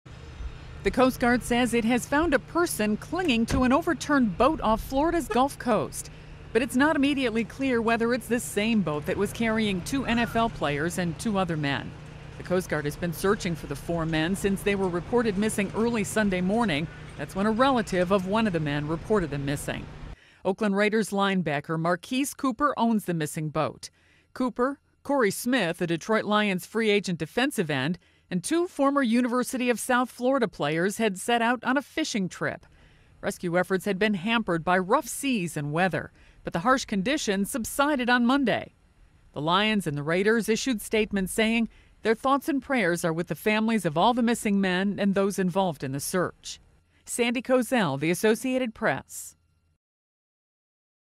Boat Found; Unsure if NFL Players Aboard
The Coast Guard says it has found a person clinging to an overturned boat off Florida's Gulf Coast but did not confirm whether it is the one missing with two NFL players aboard. (March 2)